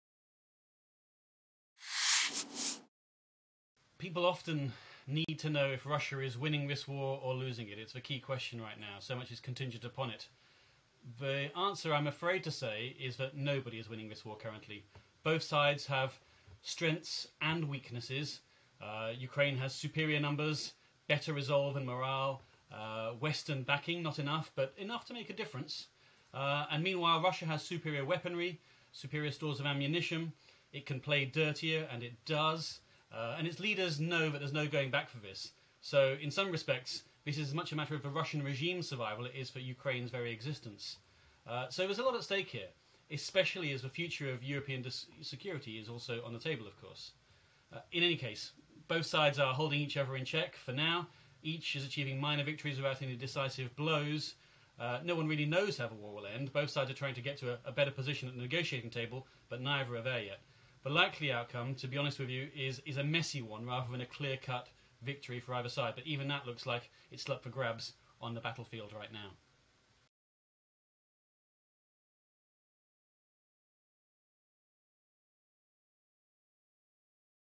Is Russia winning the war? | Explainer | Chatham House
James Nixey, director of the Russia and Eurasia programme at Chatham House outlines the likelihood or not of a victory for Russia in its war on Ukraine.

He says nobody is winning this war currently and that both sides have strengths and weaknesses. Ukraine has superior numbers, better resolve and morale and international backing, while Russia has superior weaponry and superior stores of ammunition

Both sides are holding each other in check with each achieving minor victories without any decisive blows. And both sides are trying to get to a better position at the negotiating table, but neither are there yet.

He concludes the likely outcome is messy rather than a clear cut victory for either side. But even that looks like it's still up for grabs on the battlefield right now.